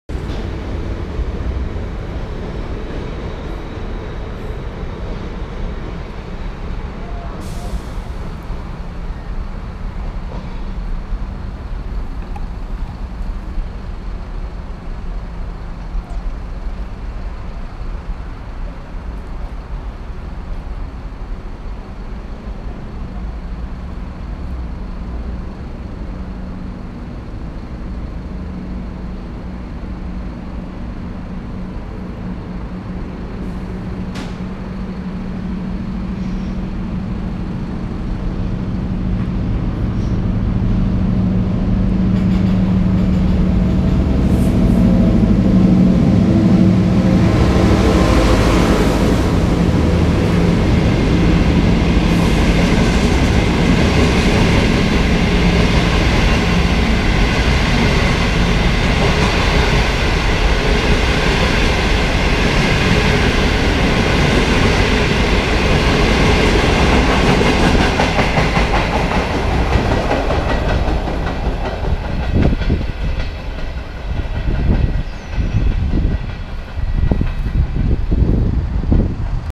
El tucumano con CKD8H0007 pasando por 3 de febrero Rumbo a Tucumán
Trenes Argentinos

Tren Línea MITRE de larga distancia

tren número 265 .. el tucumano pasando por estación 3 de febrero ( Cuidad de Buenos Aires)
Rumbo a Tucumán

a cargo de la locomotora CNR DALIAN CKD8H0007

Registro del vídeo: Día Domingo 19 de junio del 2023 por las noche a las 21:27 horas